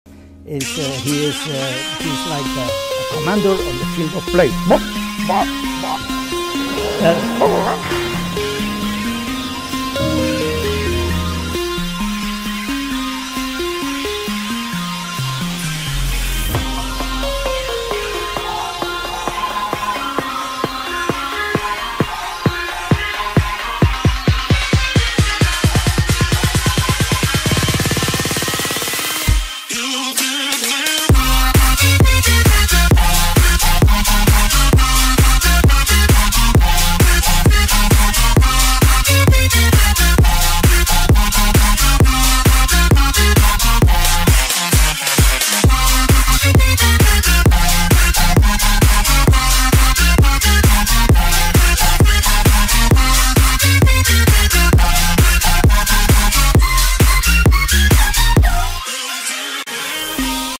Cristiano Ronaldo's commander celebration for Sepp Blatter 2013
►The GOAT: Strong Legs, Clear Mind

Cristiano Ronaldo scored in the 32nd minute of Real Madrid's match at home to Sevilla on Wednesday evening, and after the goal, Ronaldo celebrated with an apparent swipe at FIFA president Sepp Blatter.

Earlier this week, when seeming to speak about Ronaldo, Blatter was quoted as saying: "The other one, this is something else. The other one is like a commander on the field of play." Blatter also suggested Ronaldo spends more time at the hairdresser than his rival Lionel Messi.

So, after scoring his first goal of the night, a penalty, Ronaldo celebrated with a salute as any commander would do. It's hard to know for sure, but it seemed like a reference to Blatter's comments.